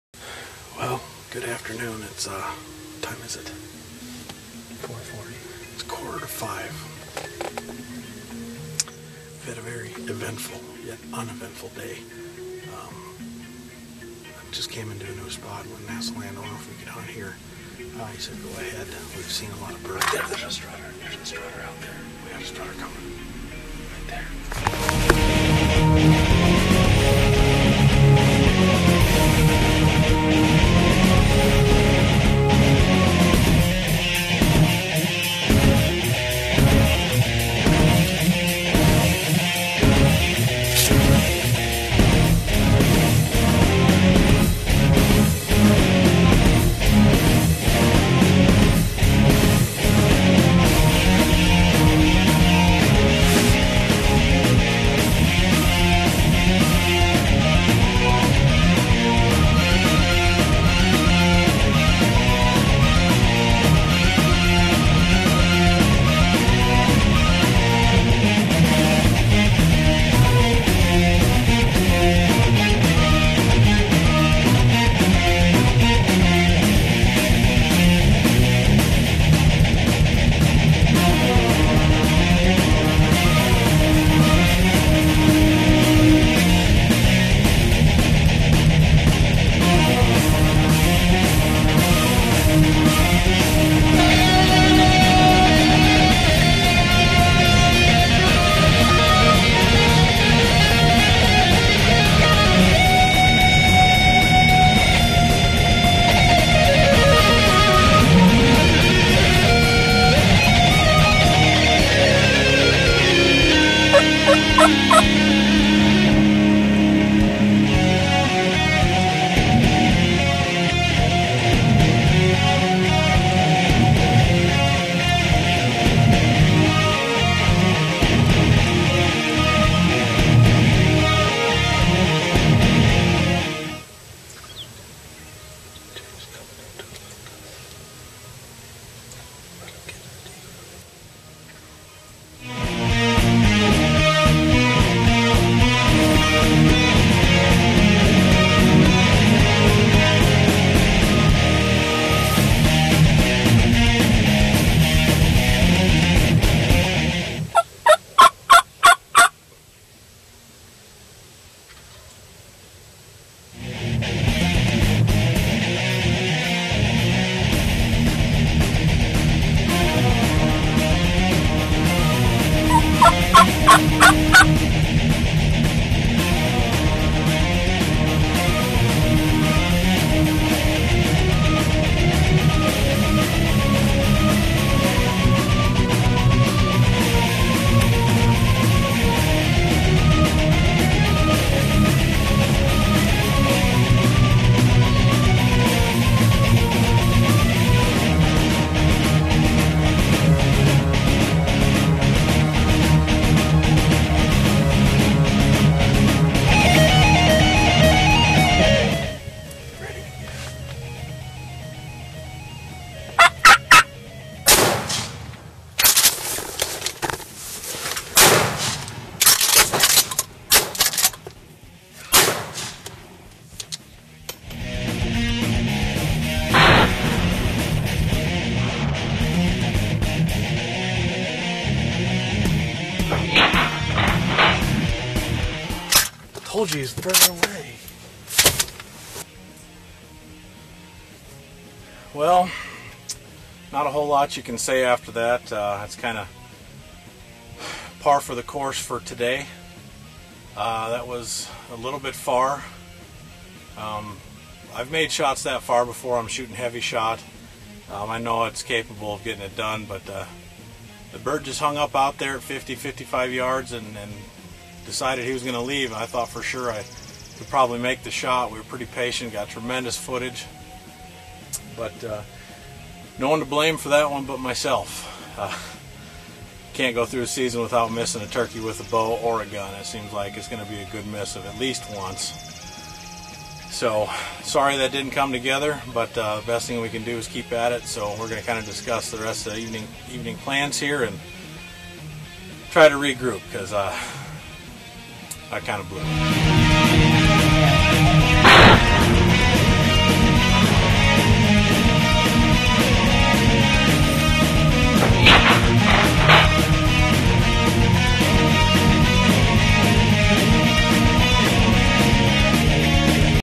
Thomas Hates Silver Platter Opportunities
On the first day of Iowa's 4th season, Thomas manages to get a big gobbler on his feet during the late afternoon... He works in perfectly and Thomas. well. just watch...